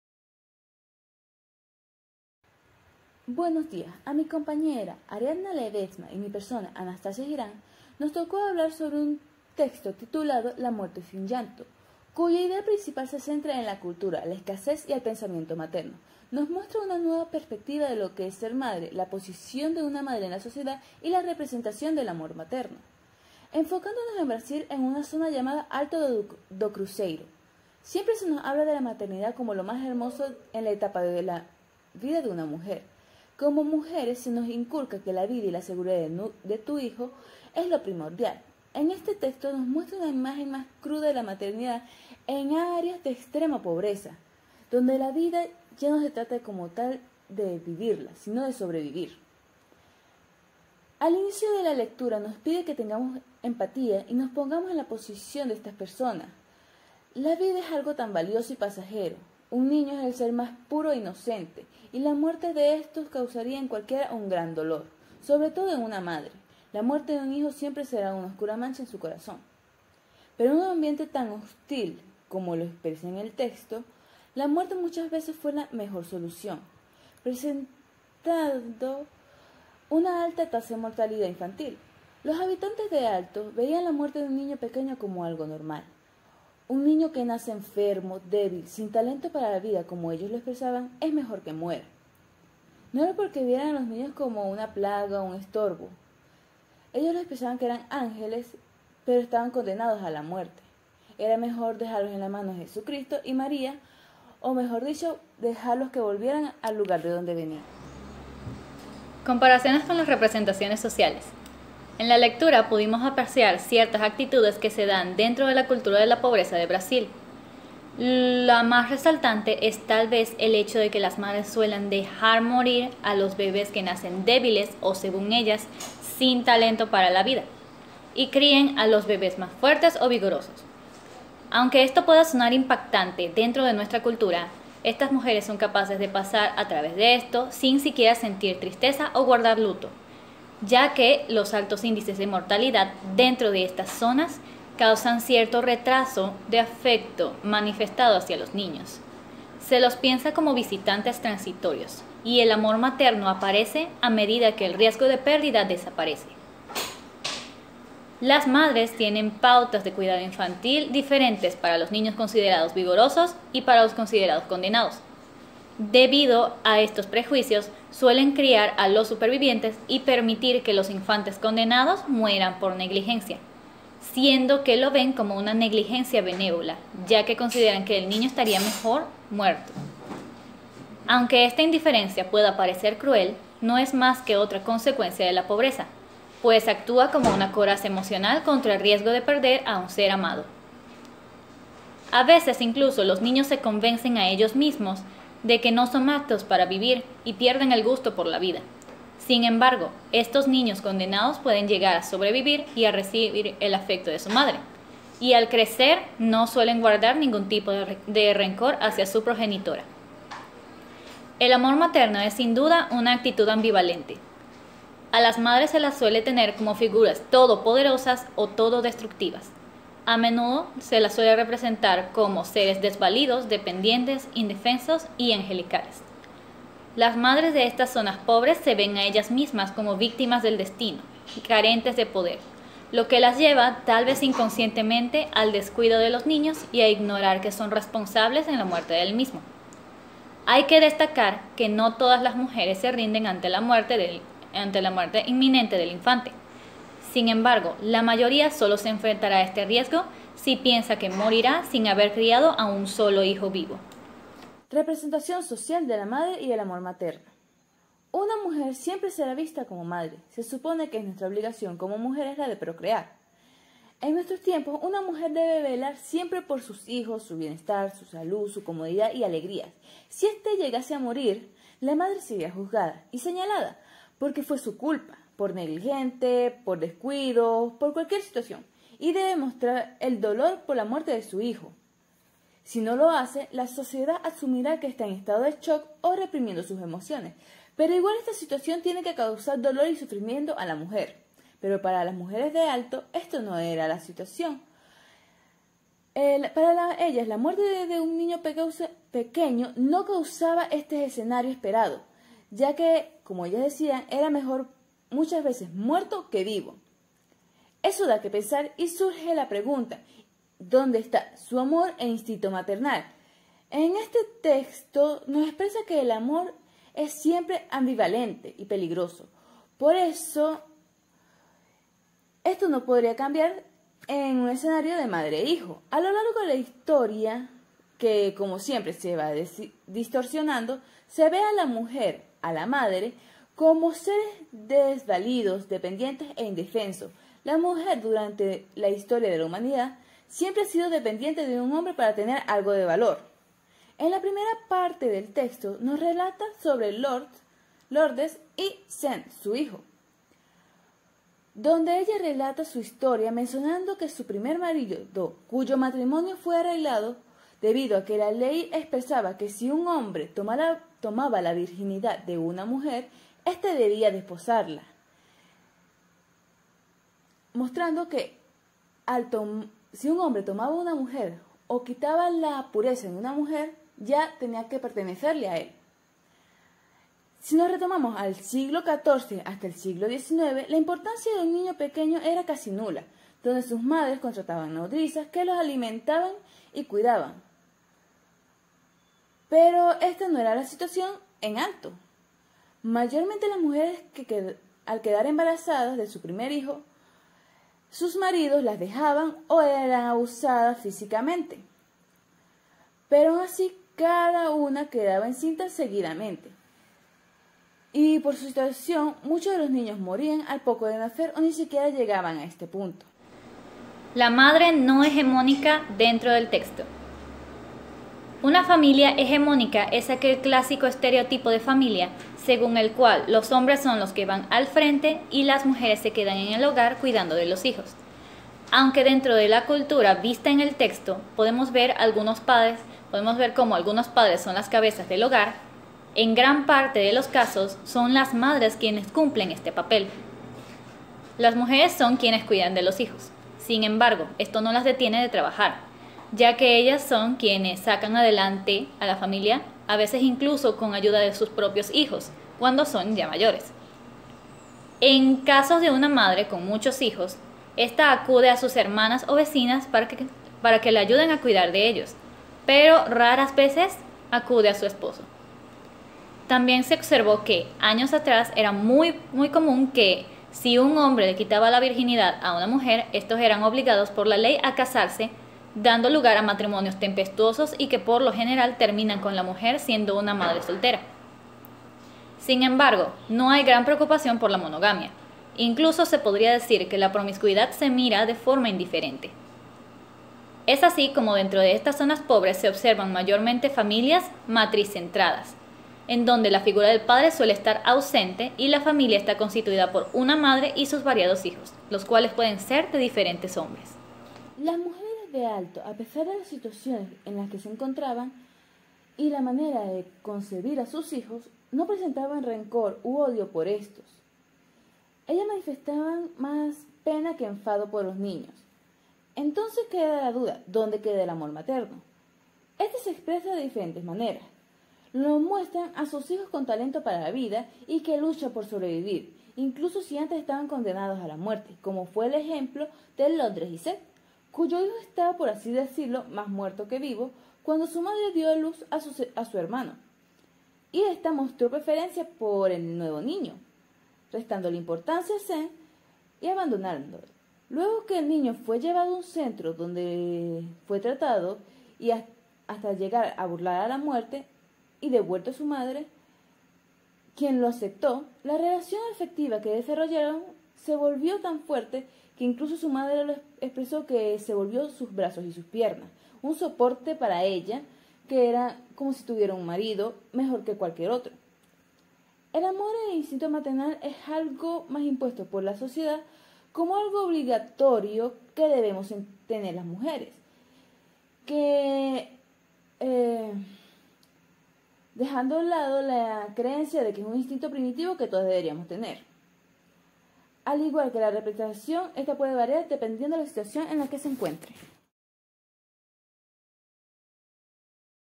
La muerte sin llanto - Nancy Scheper Hughes
Grupo #5

Tema: La muerte sin llanto
Una visión sobre el amor materno y las representaciones sociales del papel de la madre dentro de las localidades más pobres de Brasil, en donde no se lucha para vivir, sino para sobrevivir.

Integrantes:
Anastacia Giran
Ariadna Ledezma